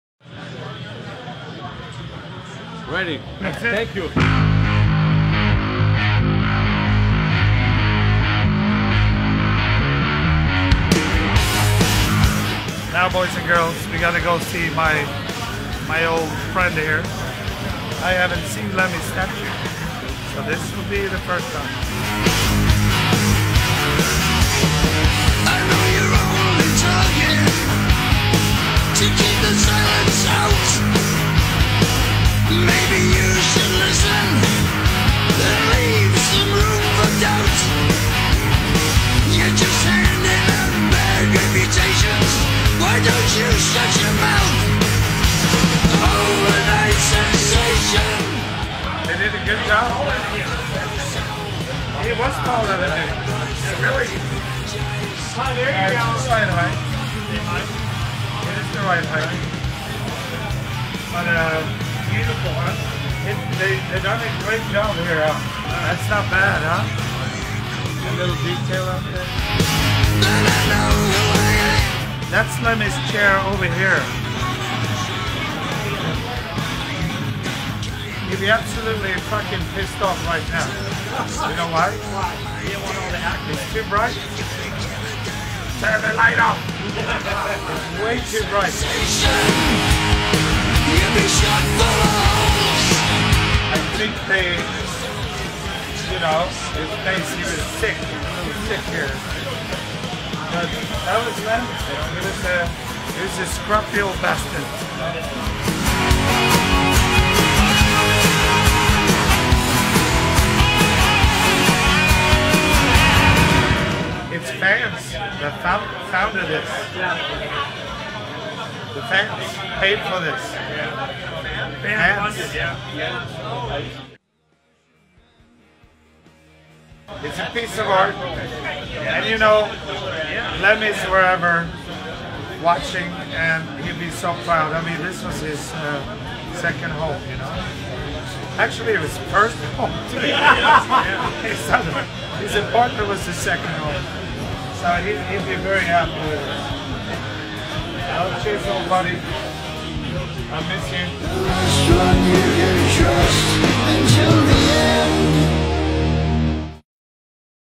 Motörhead Drummer Mikkey Dee Visit Lemmy Kilmister Statue At The Rainbow Bar
Legendary drummer Mikkey Dee recently launched a video series called A Week in Dee World, and on the most recent episode Dee visited the memorial statue of his late Motörhead bandmate, Lemmy Kilmister.